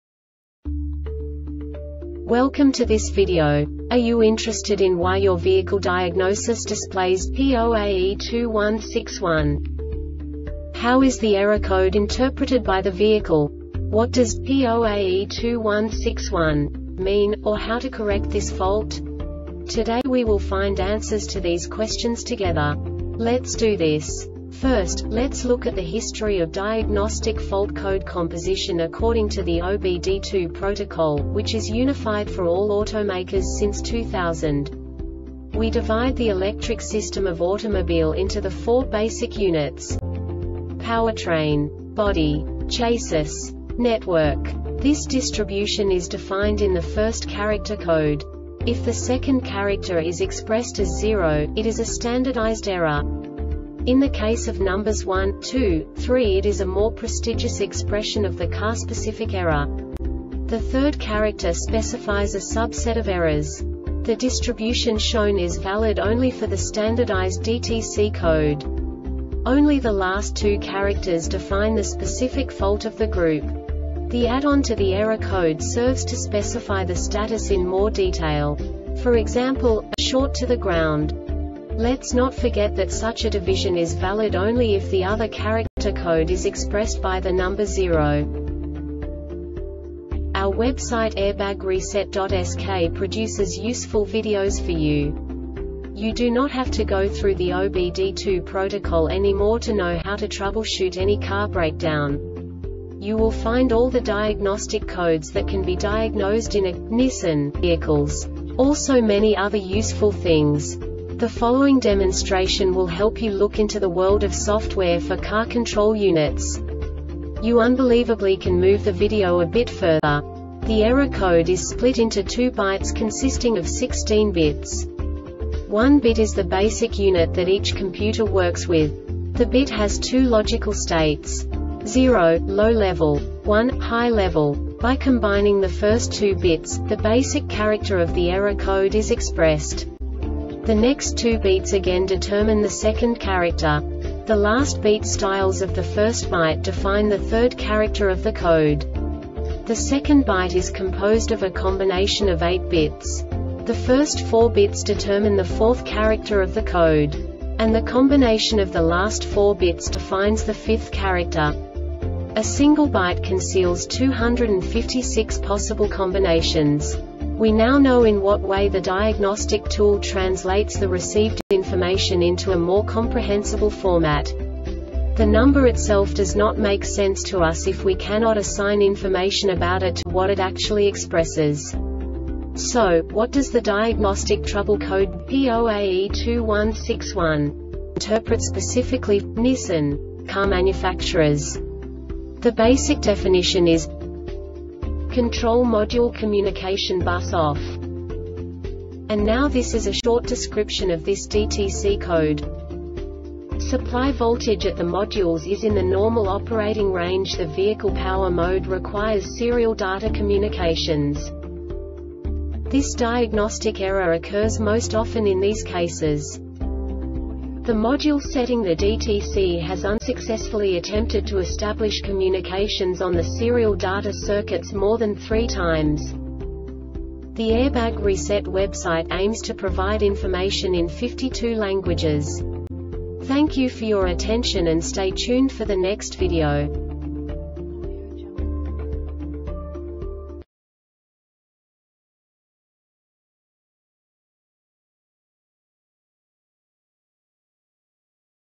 DTC Nissan P0AE2-161 Short Explanation
Contents:
0:21 Basic DTC analysis according to OBD2 protocol standard.
1:48 Insight into programming
2:58 A diagnostically understandable description of the Diagnostic Trouble Code
3:05 Basic error definition

Make:
Nissan
Code:
P0AE2 161
Definition:
Control Module Communication Bus Off
Description: